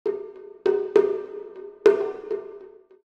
虎子石を作ってみた
謎のクリーチャー「虎子石」を作ってみた。
材料:石、漆